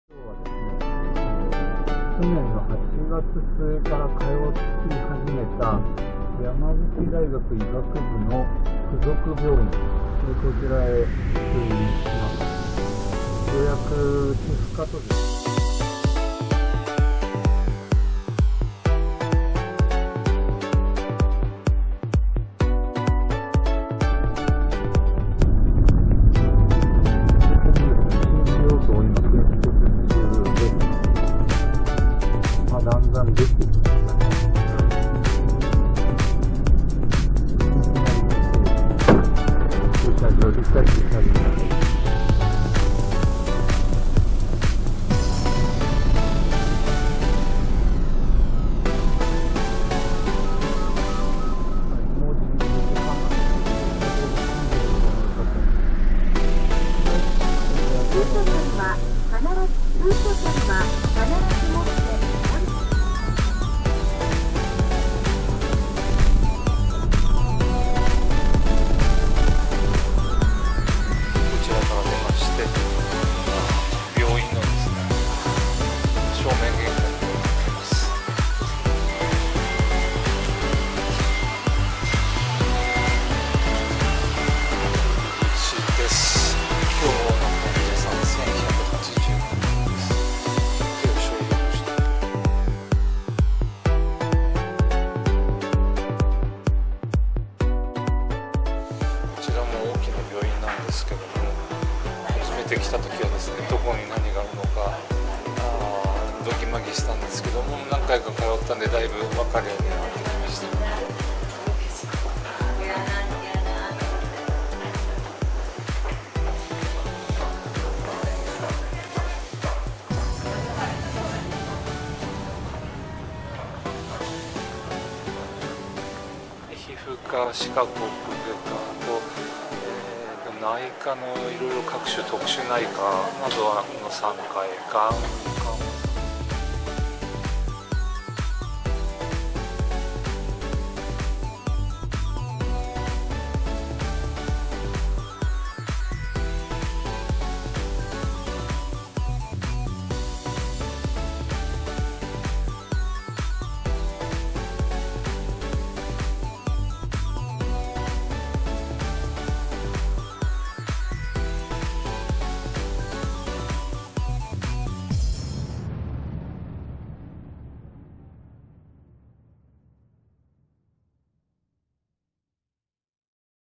山口大学病院・通院記録 No.25 2017/7/28
2015年6月に発症した謎の口内炎ですが、歯科医院の紹介で地元の総合病院を経て、現在は山口大学付属病院へと通院しています。2017年3月よりステロイド治療を開始しましたが、効果が弱く、病名や原因究明の為、山口大学附属病院の紹介で、久留米大学病院への通院を開始しました。自己免疫性の水泡症を疑って診療・検査中です。山口大学病院へは本日で25回目の通院です。動画はCorel VideoStudio で編集し、ライセンスフリーの音楽を採用しています。